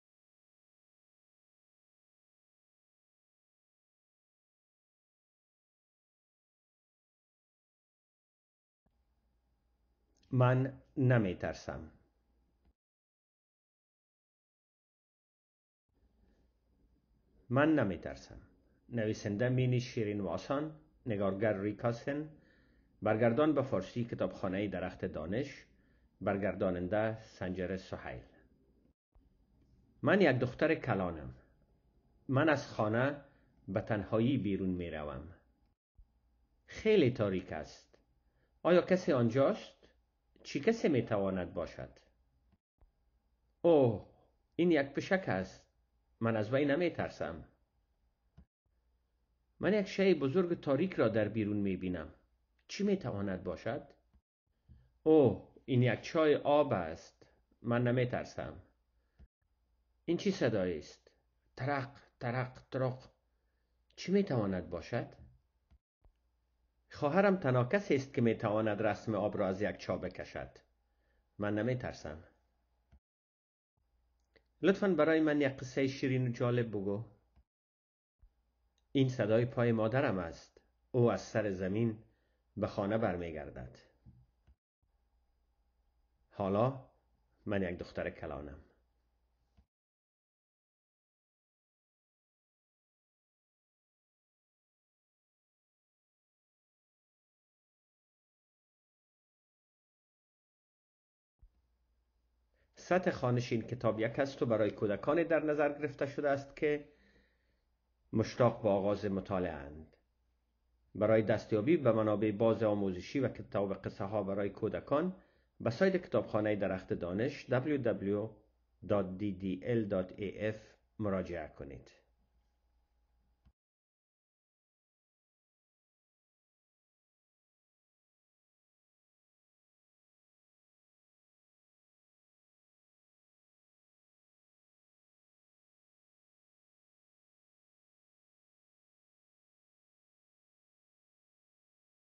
من نمی‌ترسم (شنیداری\دیداری)
دختر کوچکی در تاریکی به تنهایی بیرون می‌رود. ببینید او چه می‌یابد؟
برای دسترسی به منابع باز آموزشی و کتاب‌های قصه برای اطفال، به سایت کتابخانه درخت دانش مراجعه کنید: